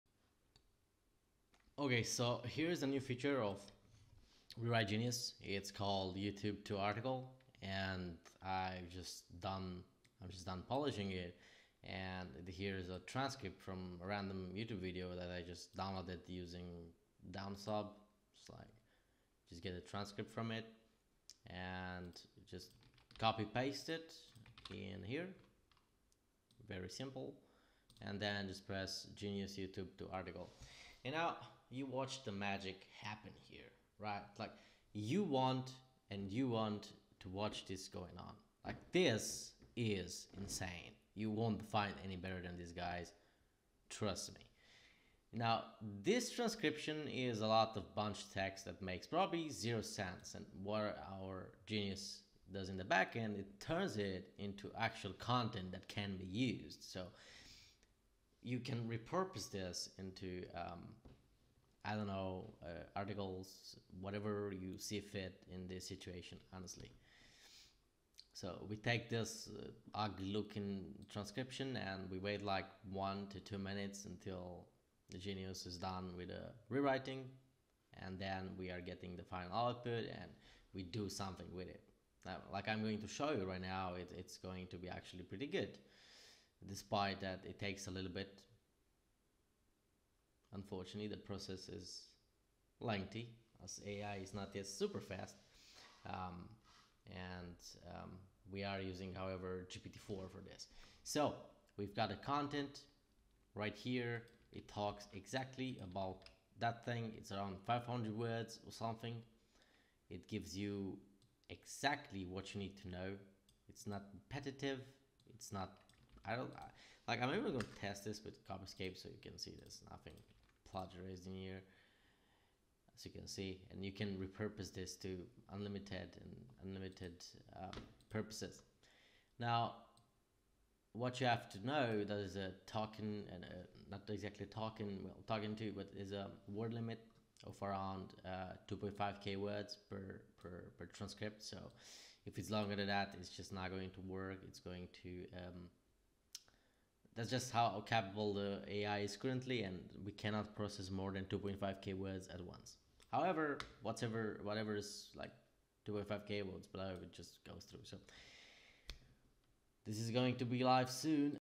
Transform Any YouTube Caption Into High-Quality Articles - Rewrite Genius AI
Feel the need to take useless YouTube caption into something greater?

Make an account, get some credits, enjoy!